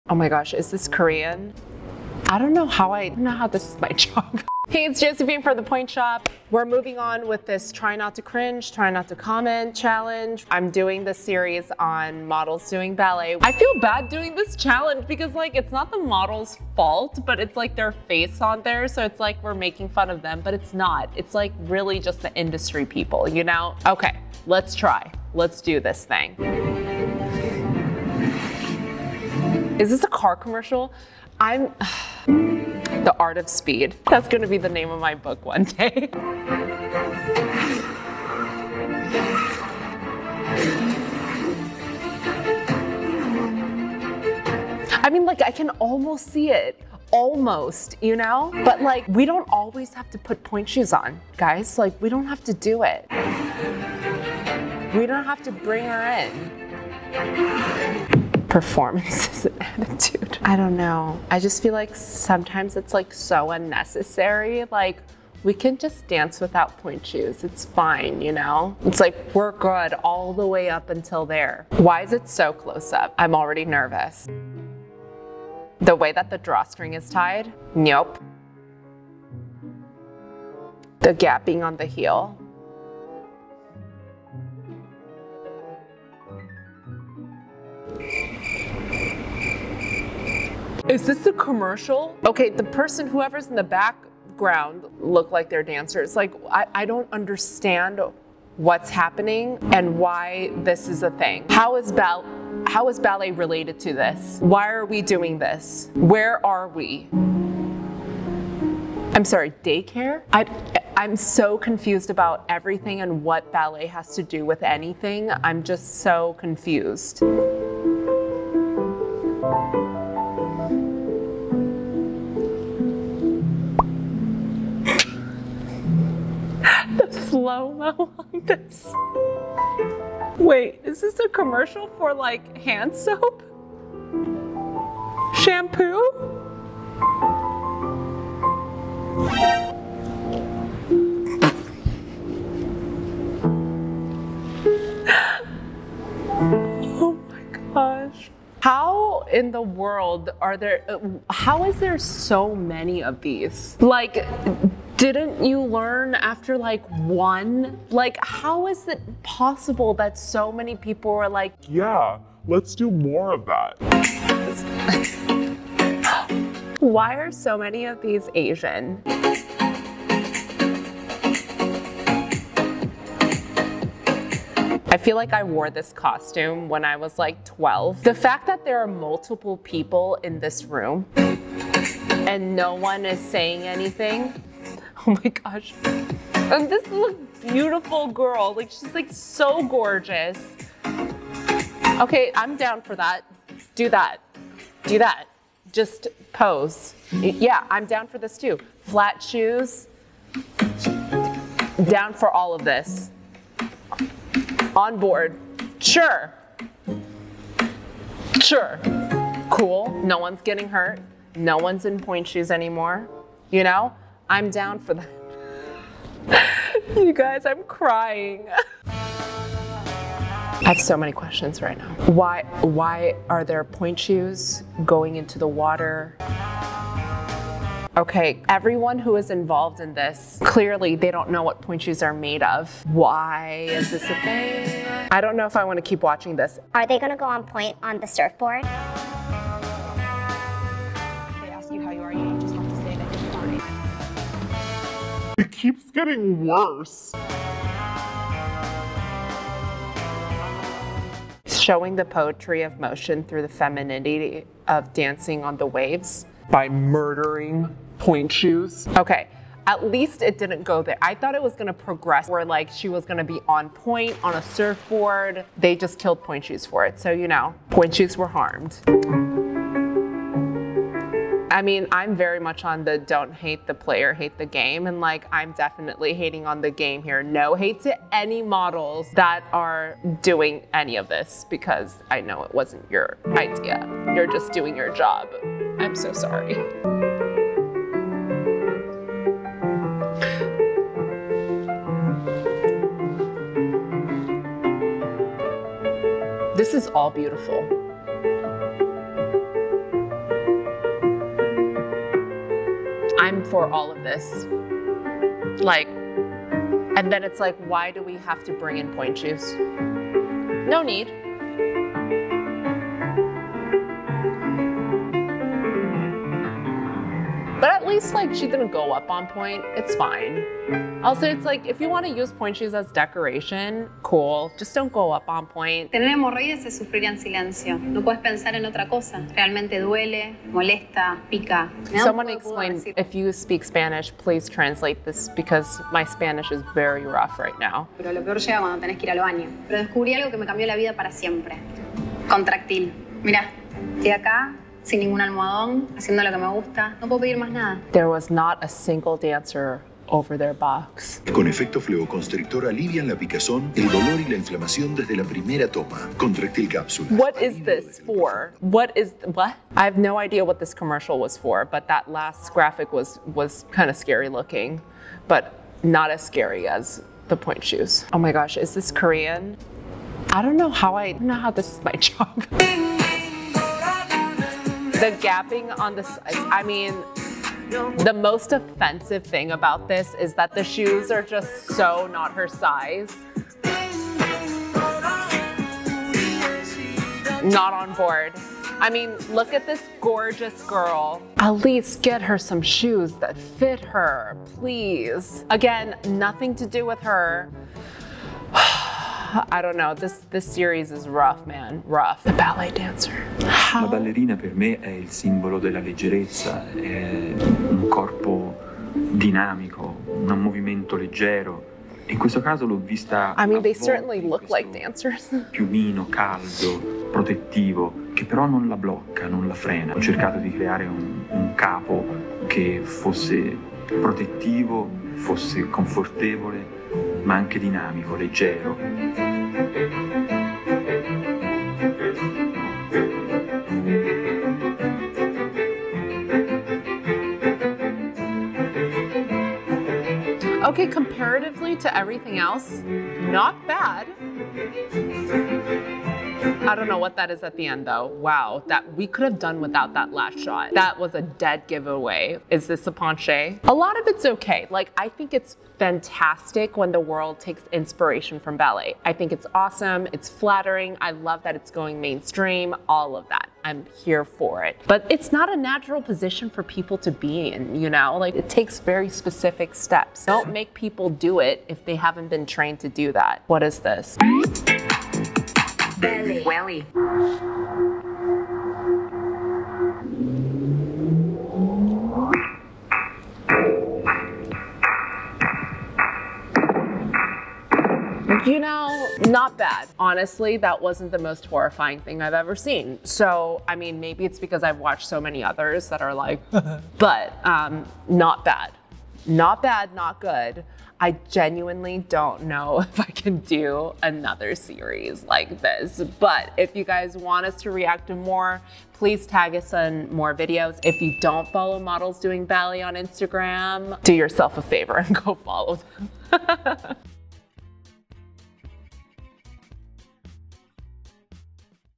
Models Doing Ballet is NOT the Problem (pointe shoe fitter reacts)
hire dancers pls
@modelsdoingballet always makes our day :-)

For more day-to-day: @josephineylee